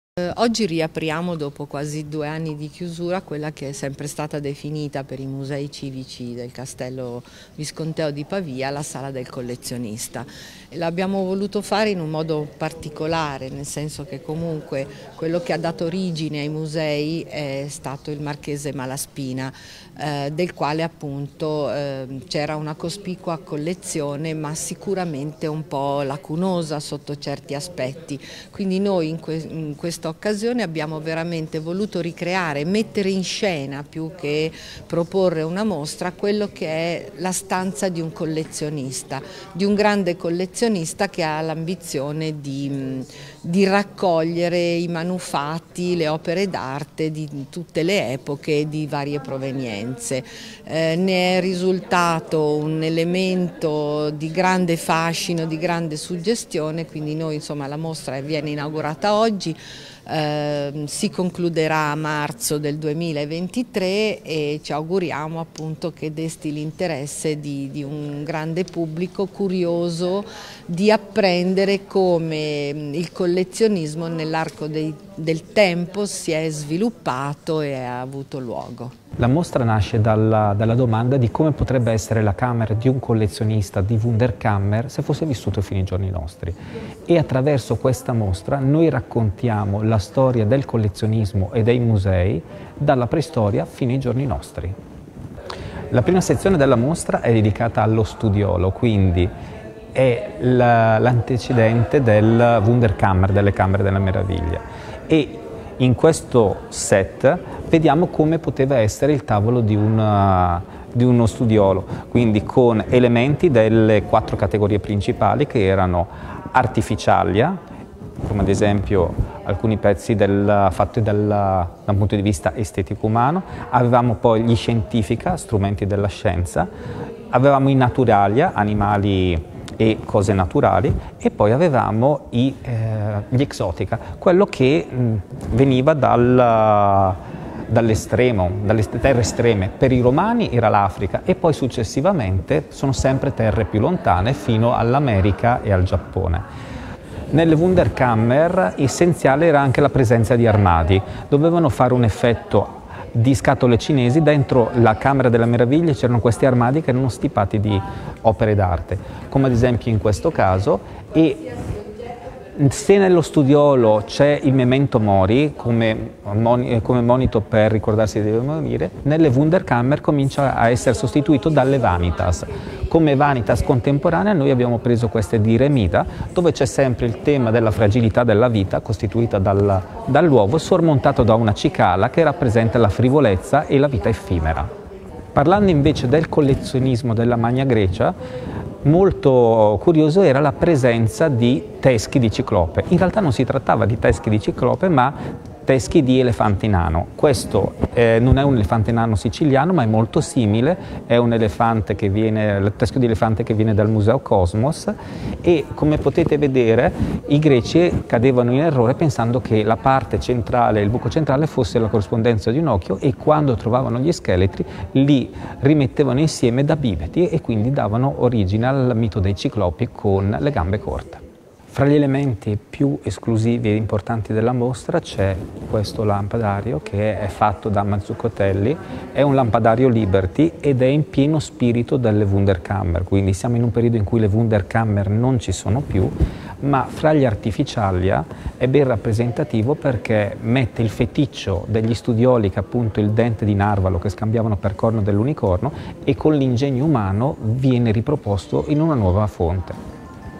Pavia - MNEMOSYNE Il teatro della memoria (20.10.22)
- Pavia - MNEMOSYNE Il teatro della memoria
Musei Civici, dal 20 ottobre al 26 marzo 2023
Tutti i giorni (escluso il martedì), dalle ore 10 alle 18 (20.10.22)